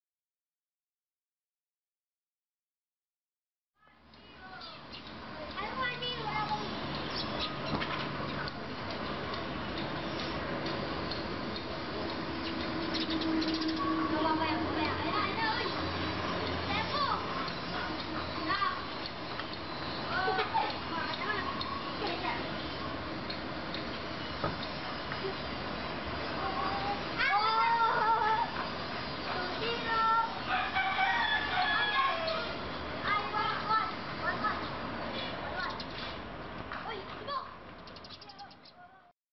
Homemade Sparrow Trap FREE Build Plans
Video showing a trapped sparrow inside the homemade bird trap.